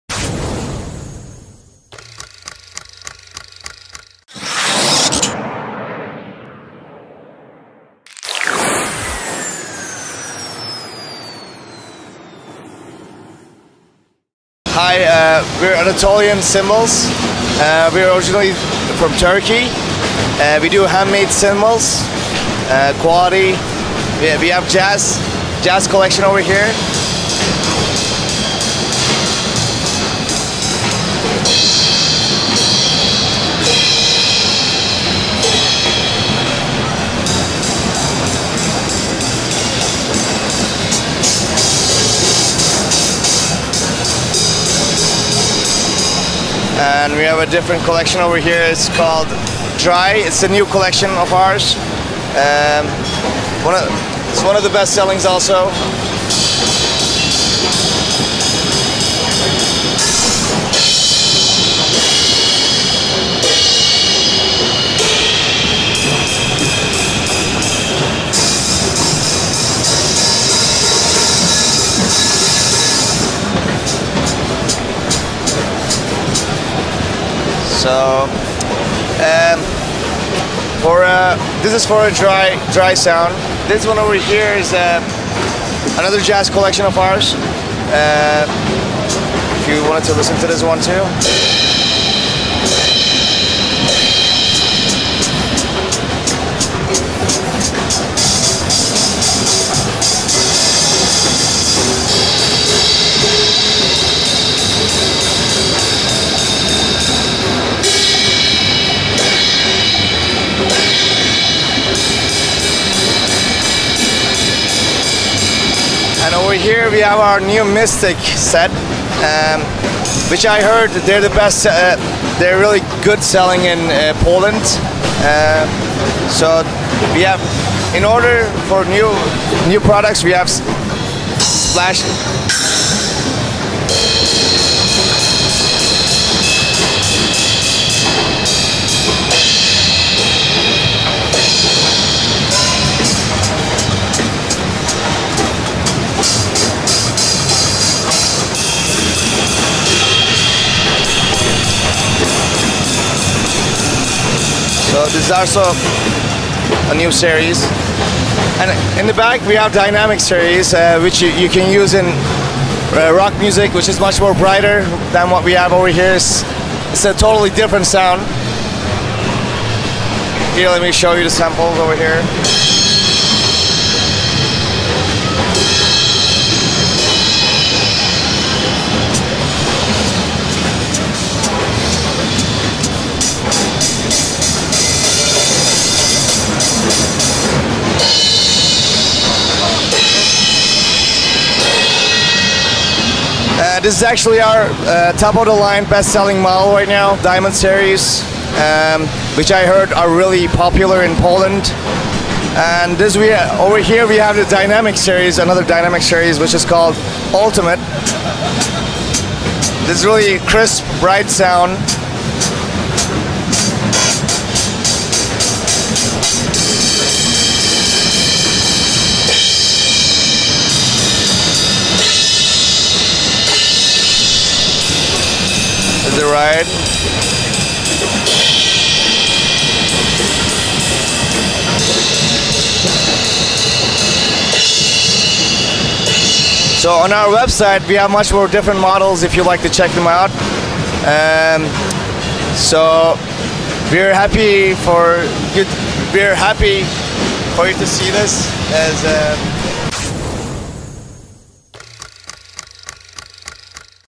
BeatIt at NAMM Show 2017: Anatolian Cymbals Booth
Here is a video report from the Anatolian Cymbals booth at NAMM Show 2017. Enjoy!